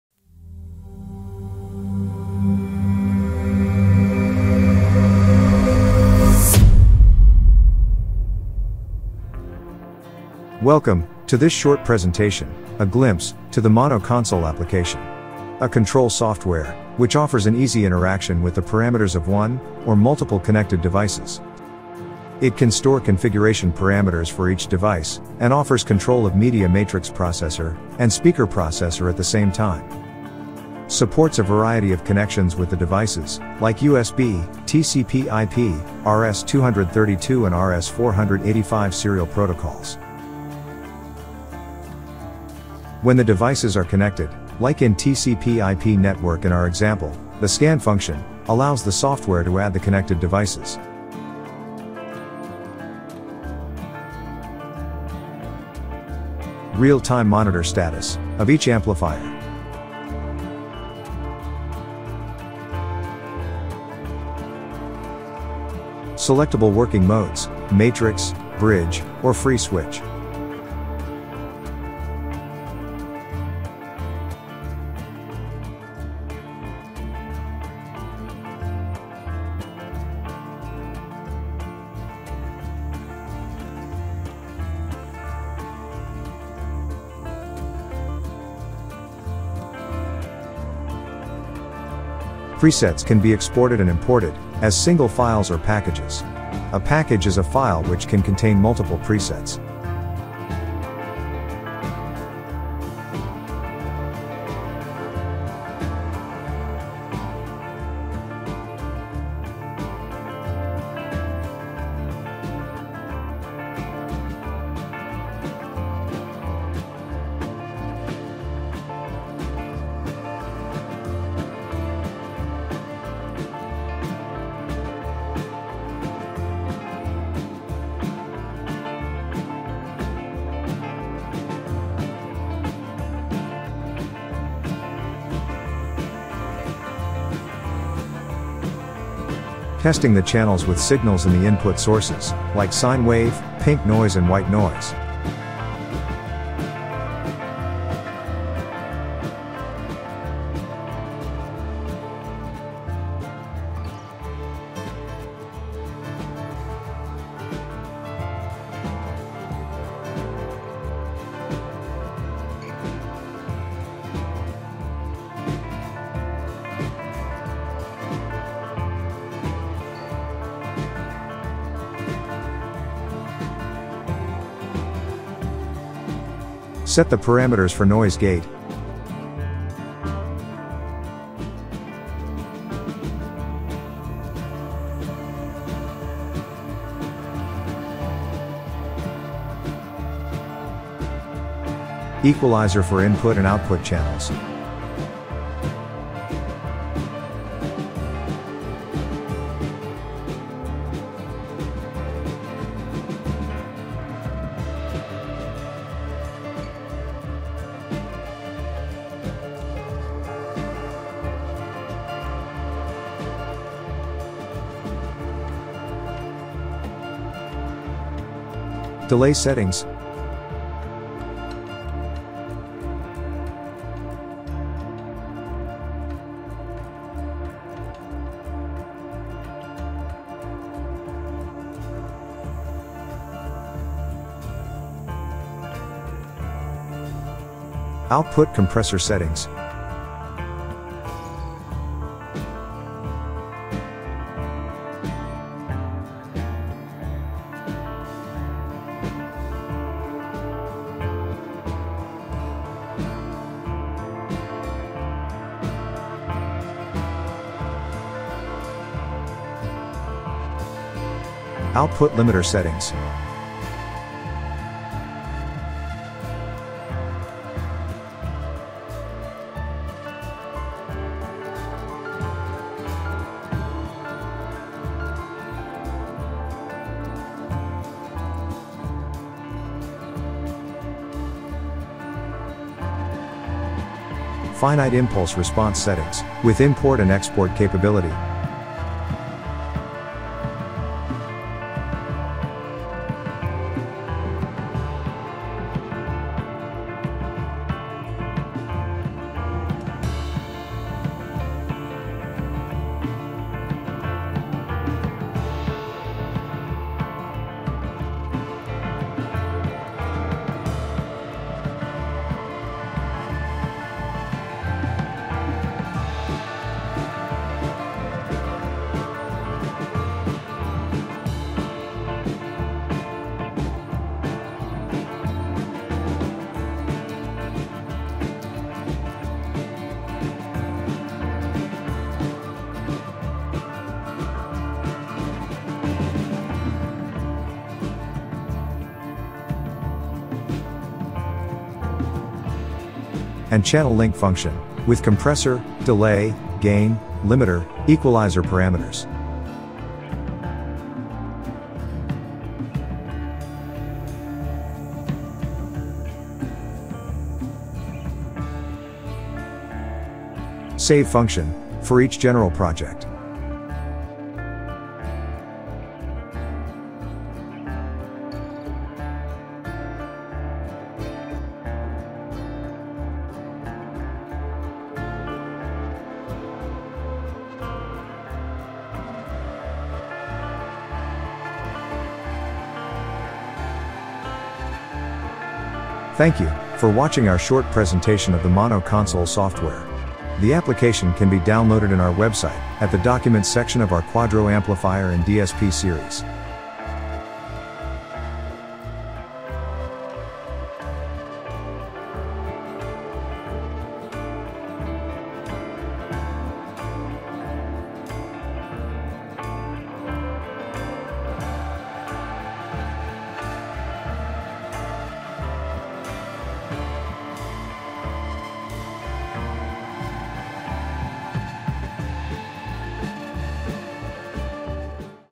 MonoConsole Software short presentation
Mon Console software can be used for controlling the amplifier parameters remotely, via Ethernet network, serial, or USB connection.